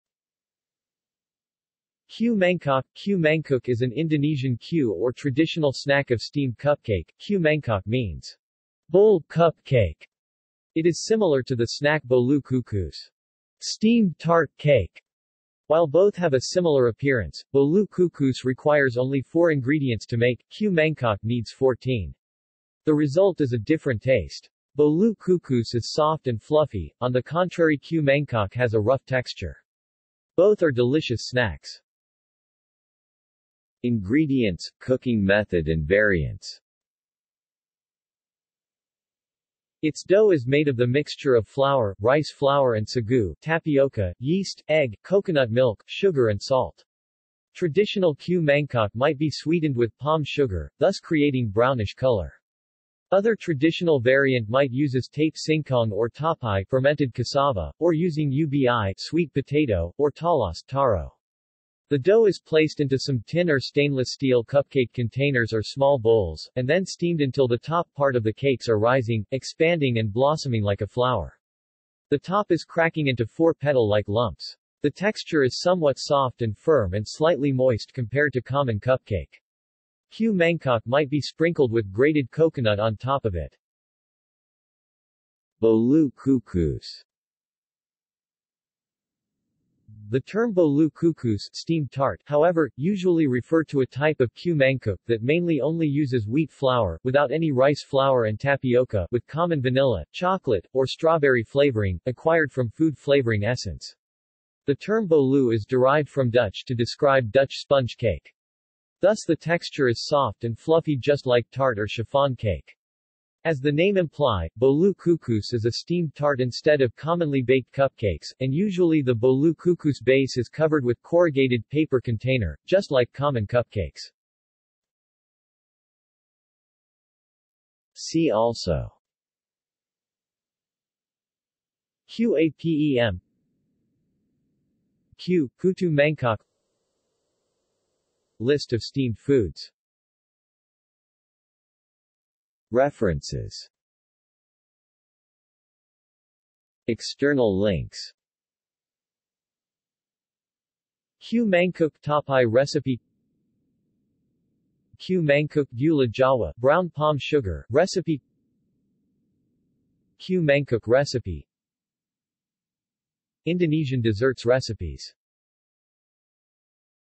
Kue mangkok | Wikipedia audio article
This is an audio version of the Wikipedia Article:
Kue mangkok

SUMMARY
Kue mangkok, kue mangkuk is an Indonesian kue or traditional snack of steamed cupcake. kue mangkok means "bowl/cup cake". It is similar to the snack bolu kukus ("steamed tart/cake"). While both have a similar appearance, Bolu Kukus requires only four ingredients to make, Kue Mangkok needs fourteen. The result is a different taste. Bolu Kukus is soft and fluffy, on the contrary Kue Mangkok has a rough texture. Both are delicious snacks.